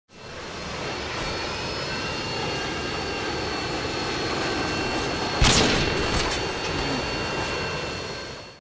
Hitmaker #15
Gay Airborne Hitmarker On Quarry And Would Have Been Nice If I Got The Guy.